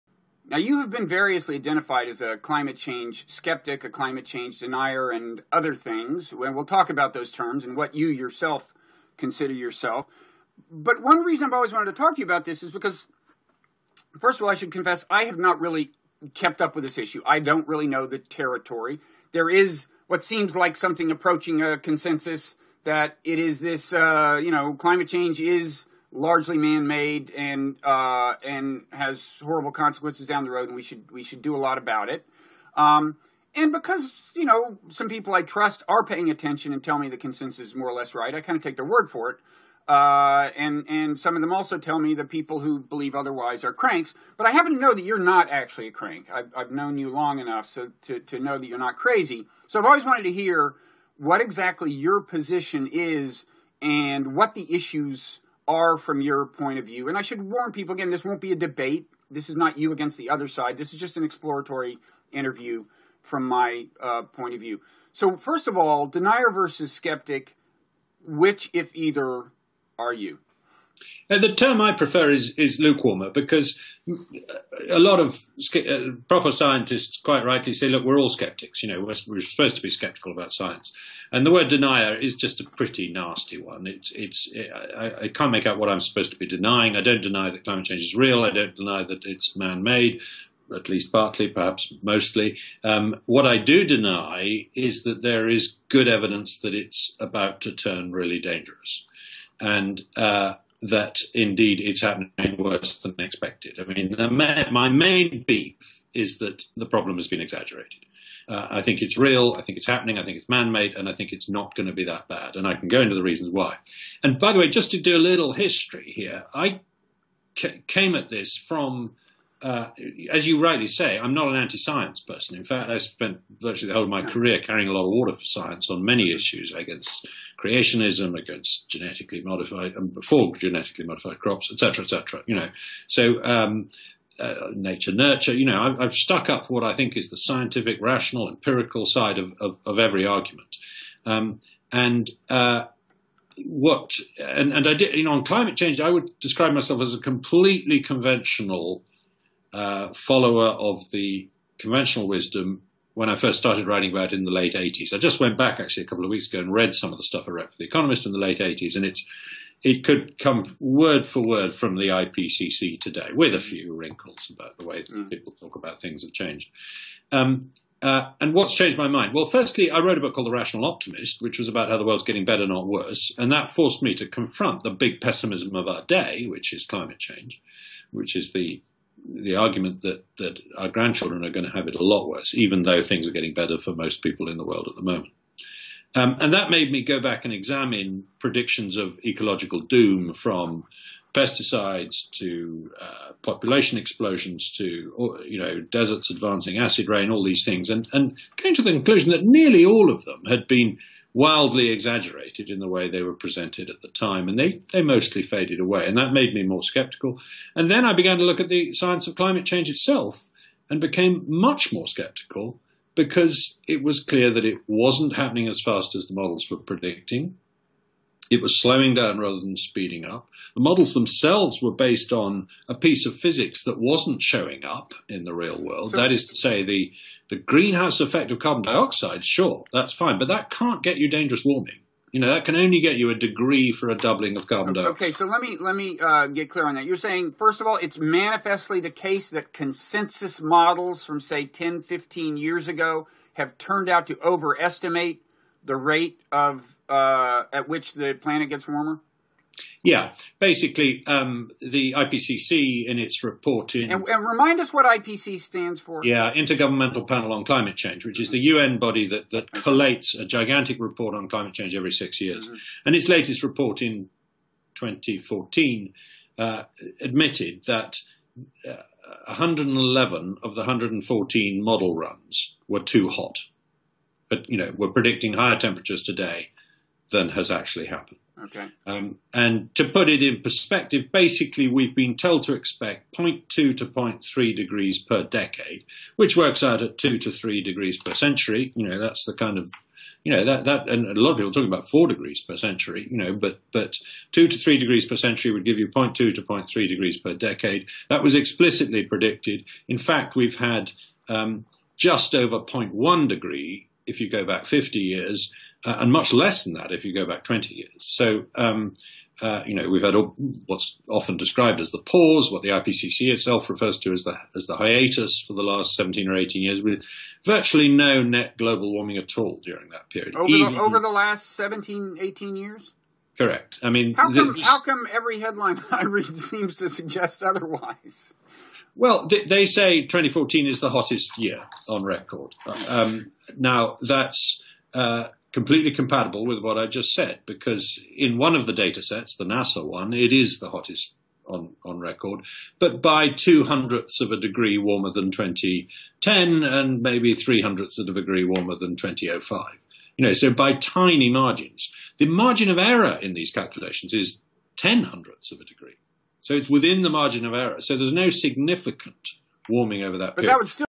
The case for calming down about climate change
Robert Wright speaks with Matt Ridley, who rejects the conventional wisdom on global warming.